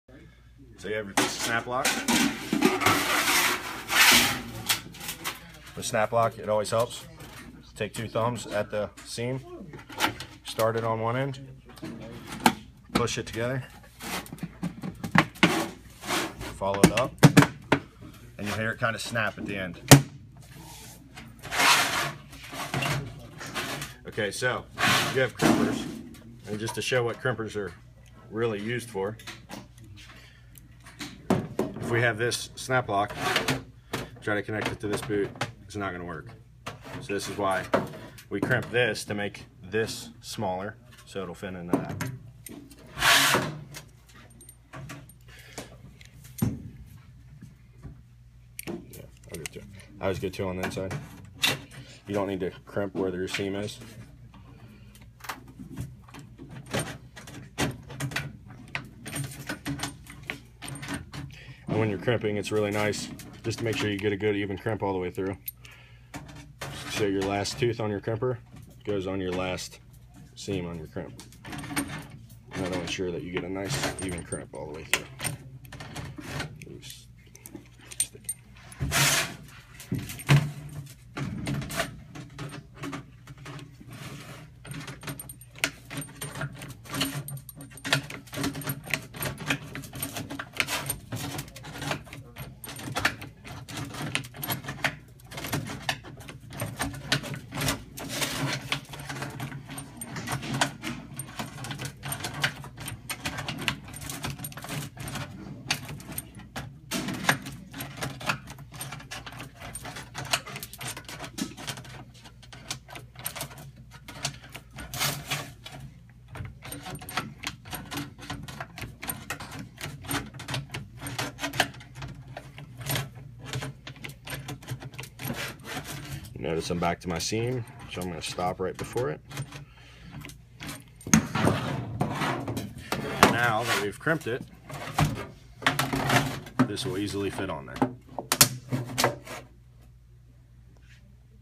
HVAC How to Crimp Round Ductwork
Anthony Hoekstra showing how to Crimp Snap Lock (Round Duct)